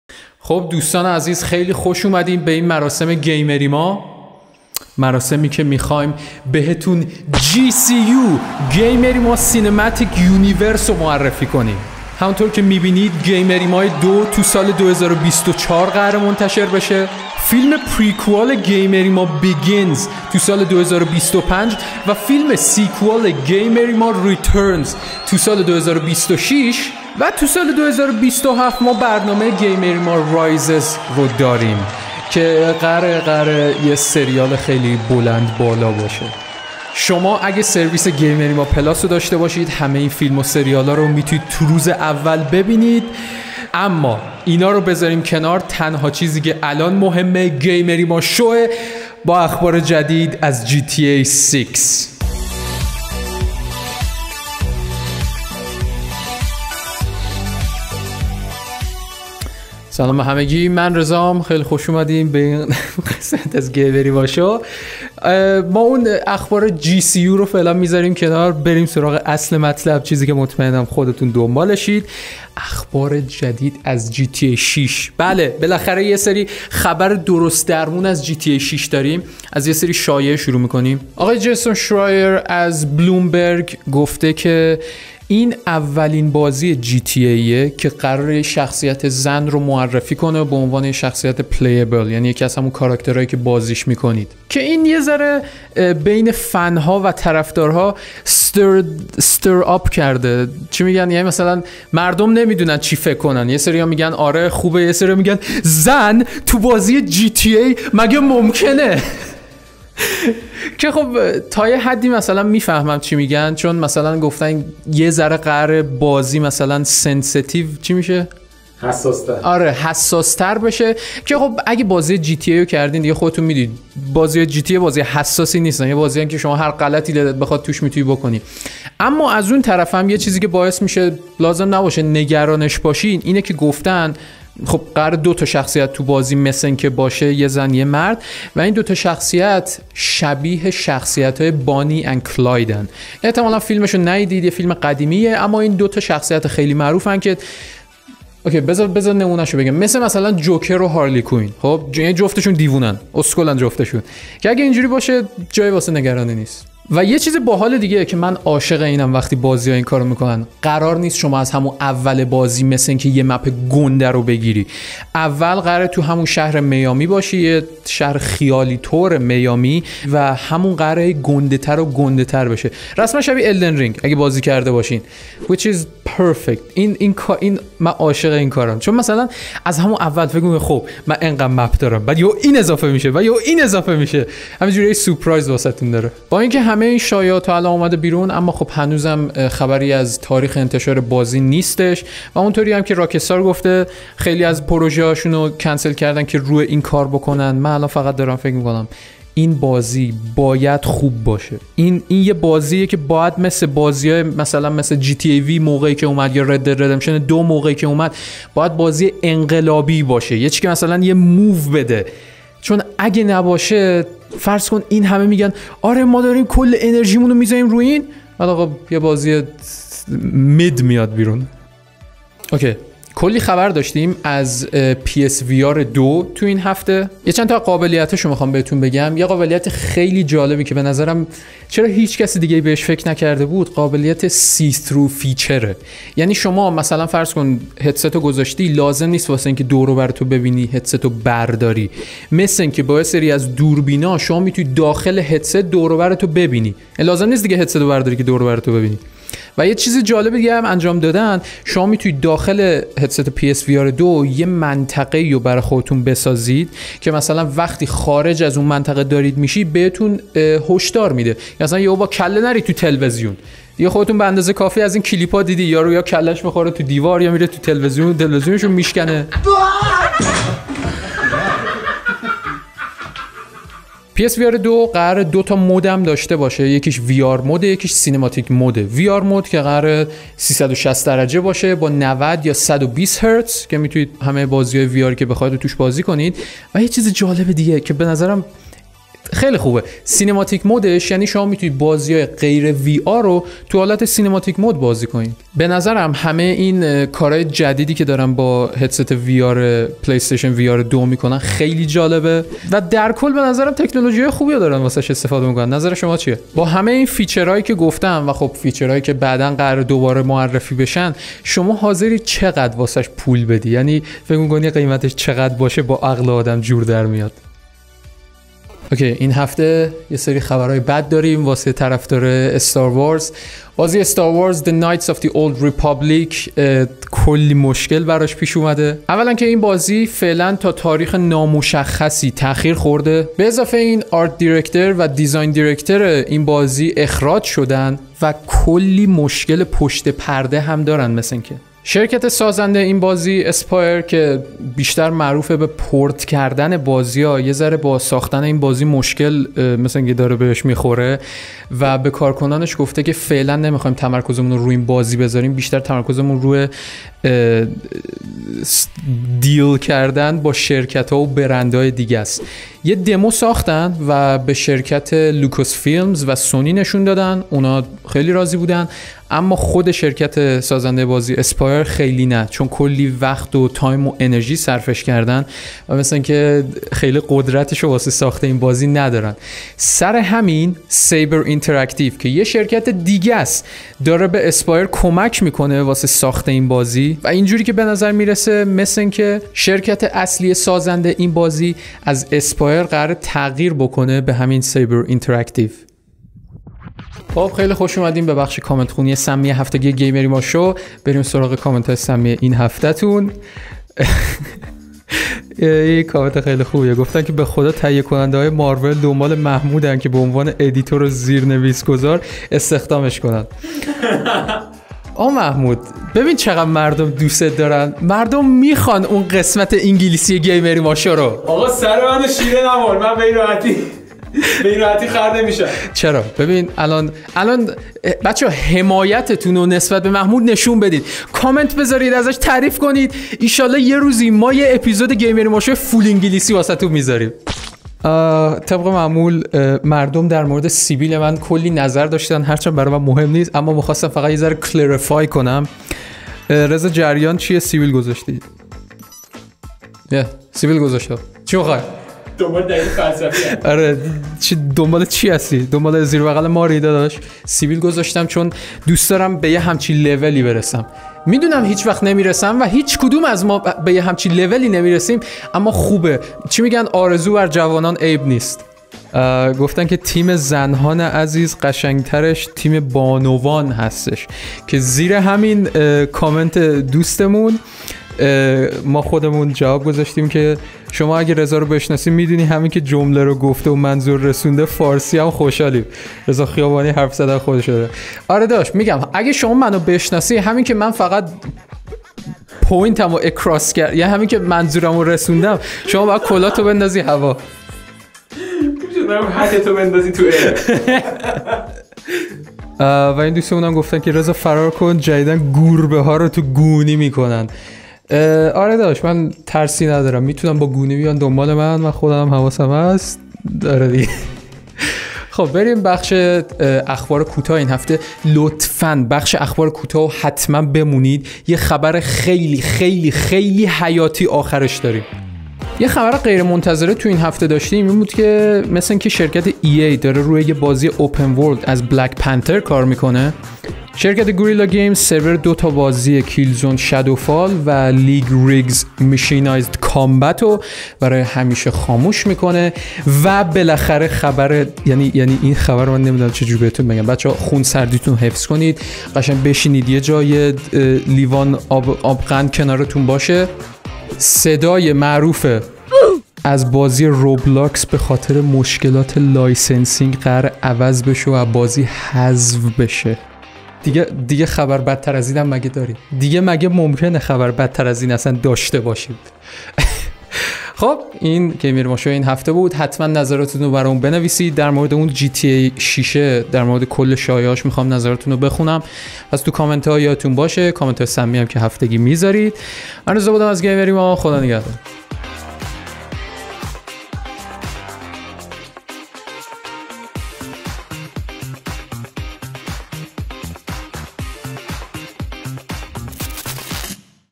همه چیز درباره جی تی ای 6 | GAMERIMASHOW
این گیمریماشو از اون خوباست. ببینین کی گفتم!

لایک و سابسکرایب یادتون بره!

دمتون گرم، خیلی دوستون داریم.

وبسایت رسمی: Gamerima.com

اخبار هفتگی گیم
Gaming News
گیمریماشو

CHAPTERS
0:00 GCU
0:50 لایکو بزن بریم سر اخبااار
1:12 GTA VI شایعه ها و اخبار لو رفته از
3:43 PSVR 2 اطلاعات جدید از
5:19 Knights of the old republic مشکلات توسعه بازی استاروارز
6:37 کامنت خونی سمی
8:45 اخبار کوتاه
9:43 تا هفته بعد، خدانگهدار